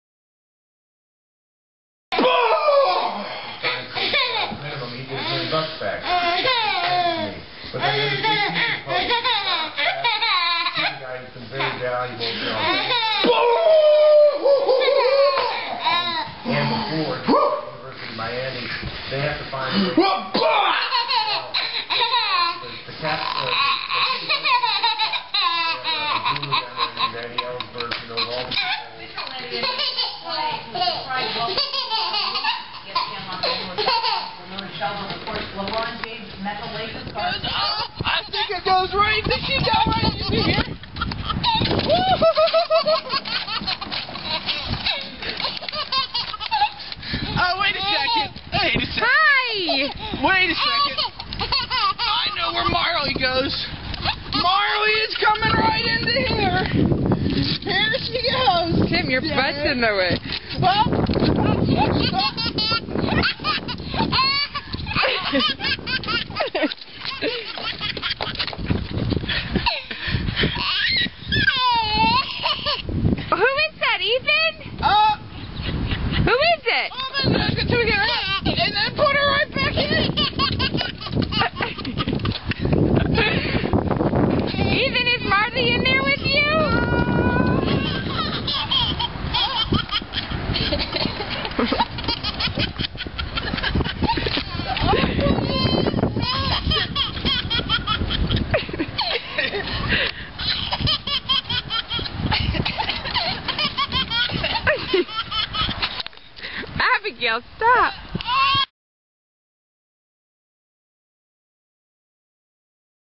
Baby Ethan laughing at dog!!!
Baby laughing! Ethan in hysterics over Marley getting his wagon.